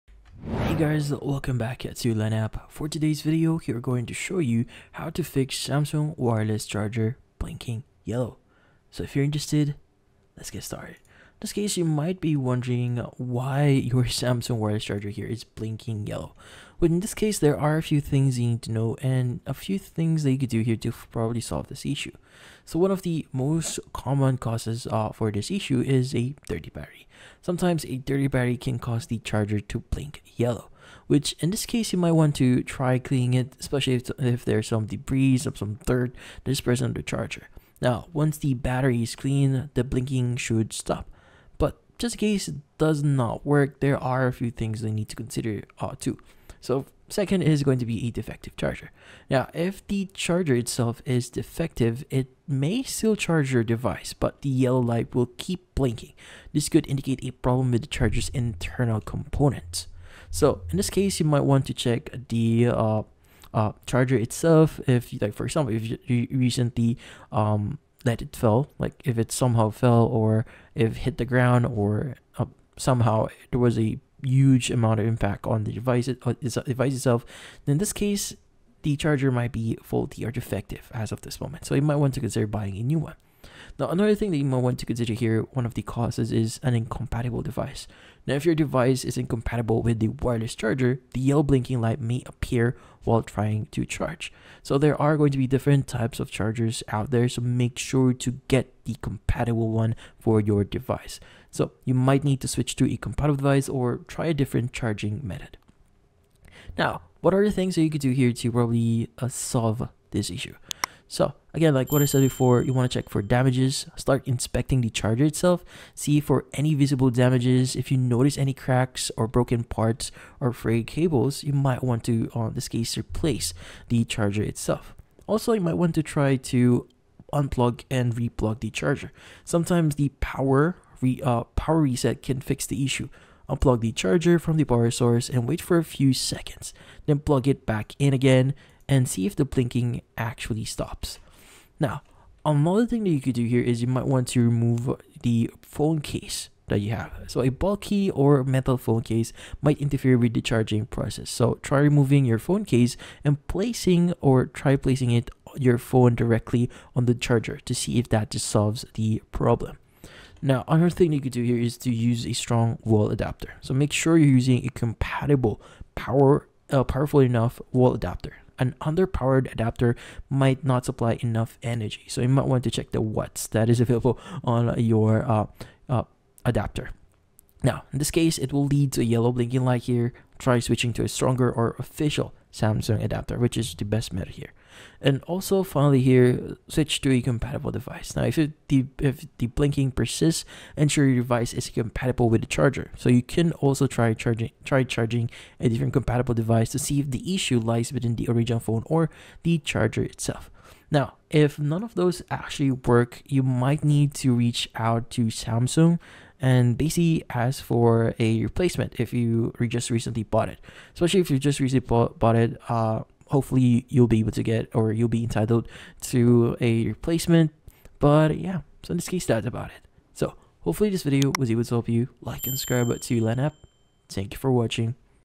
How To Fix Samsung Wireless Charger Blinking Yellow (2025)
If you want to be able to fix your samsung wireless charger blinking yellow, then this video will be perfect for you!